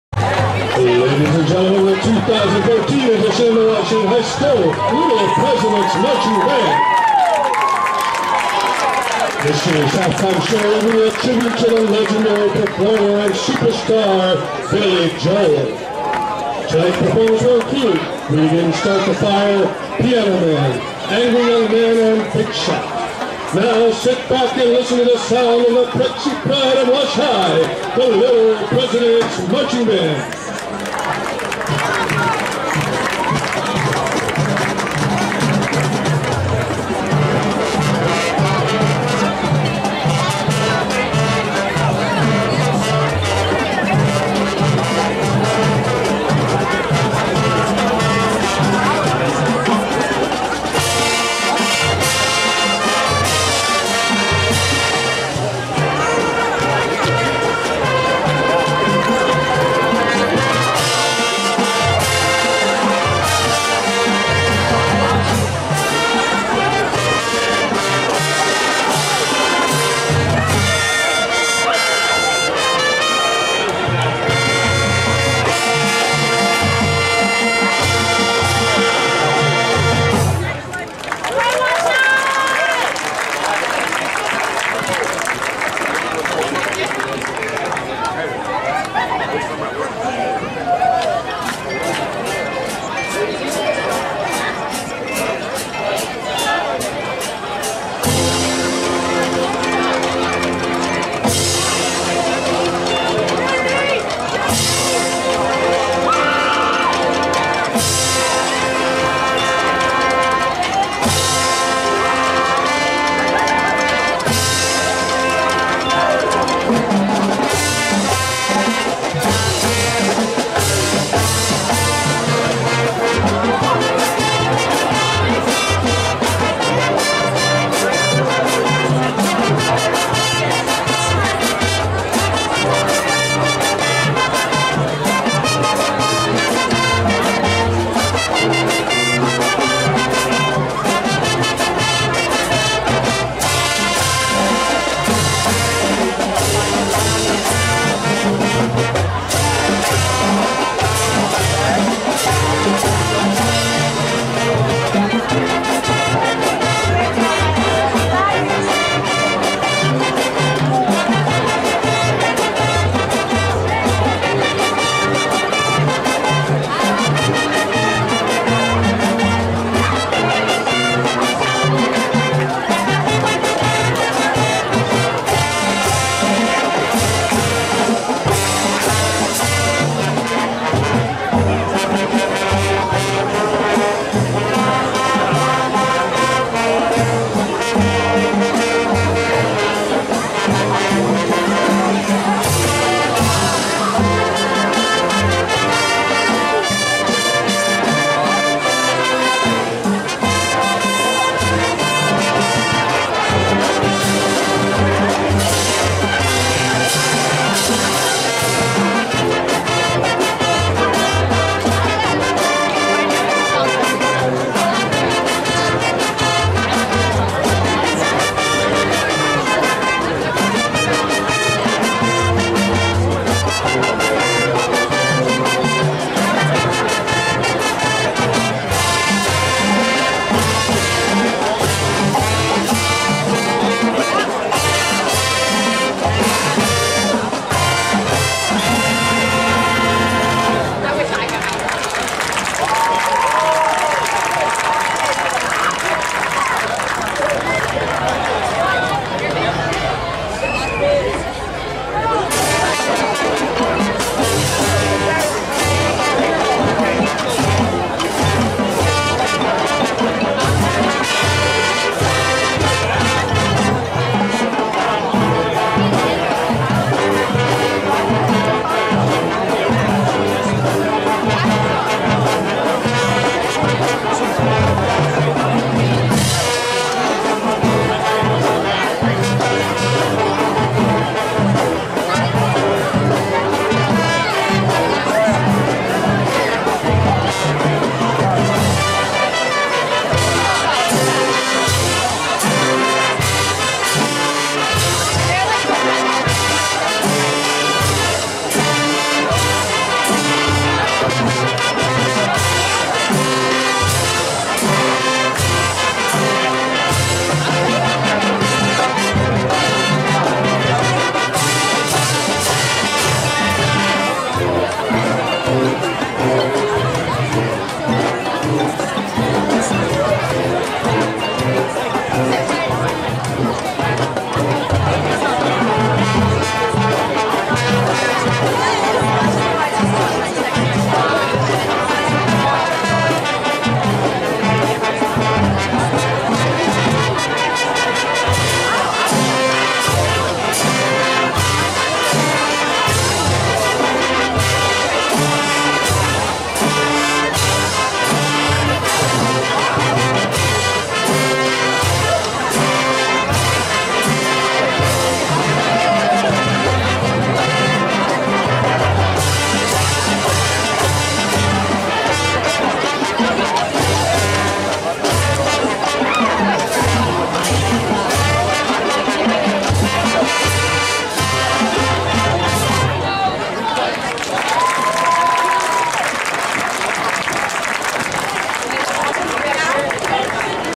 Wash High Prexie 2014 half-time show
The Wash High Little Presidents marching band performs their half-time show at the Wash High stadium on August 20, 2014.
Director: David Dayton
Assistant director: Mark Poole
Drum major: Tyler Funk
Drum captain: Nathan Staso